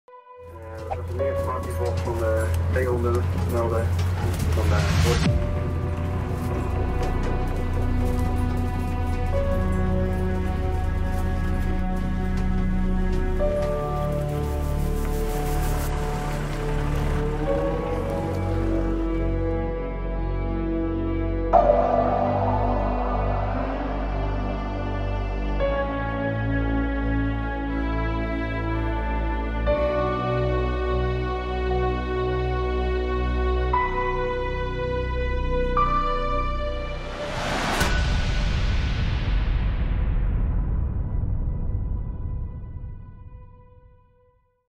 Val op duinpad zuidduin Katwijk
Vandaag (zondag) werd de bemanning rond half twaalf gealarmeerd voor het verzoek van de meldkamer ambulance Hollands-Midden om de ambulancedienst te assisteren in de Zuidduinen van Katwijk. Hier was een man in een rolstoel van het pad afgeraakt en ten val gekomen. Omstanders hadden de man al overeind geholpen en na controle van het ambulancepersoneel is de man in zijn rolstoel weer terug geholpen op het pad.